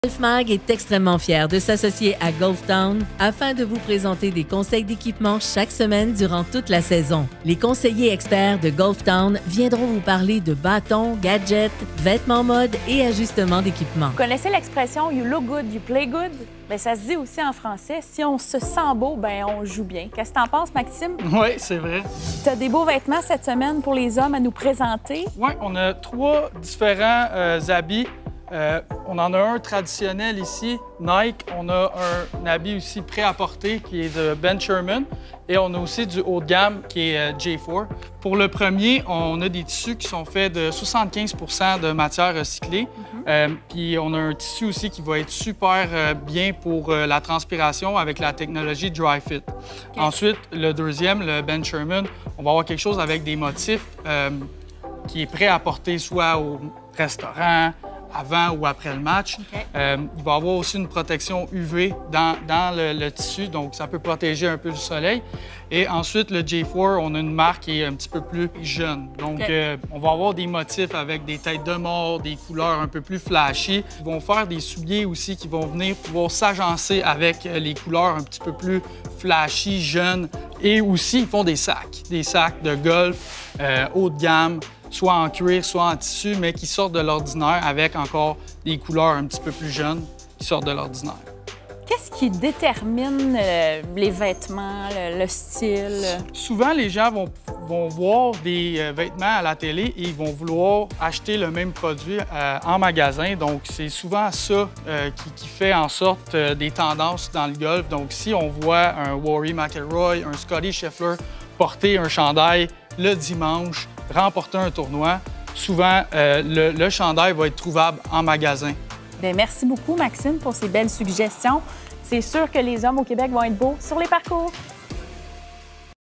Chronique Golf Town 2 Vêtements pour homme
Mamix nous parle des différents modèles de vêtements pour homme.